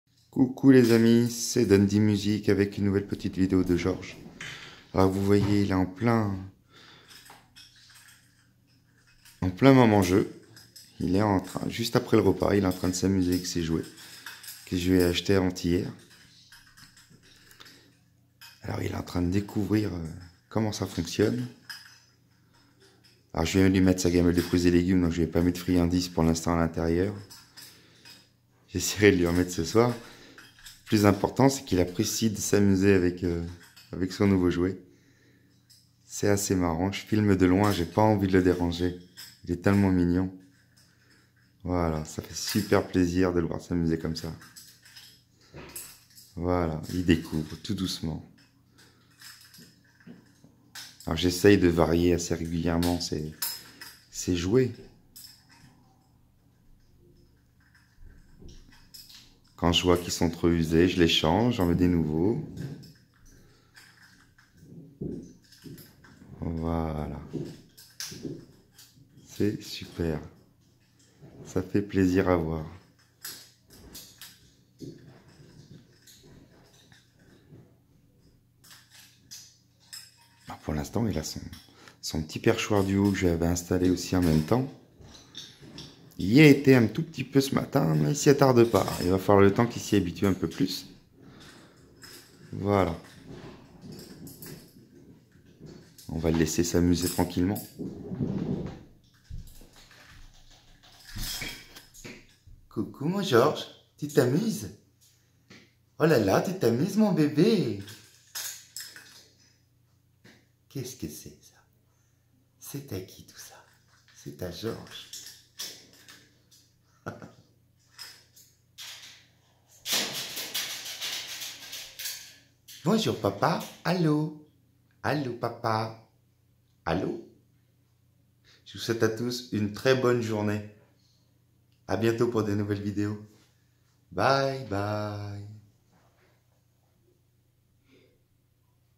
Perroquet eclectus ( découvre son nouveaux jouet )
Bonjour a tous ,nouvelle video de mon perroquet eclectus qui découvre son nouveaux jouet bonne journée à tous.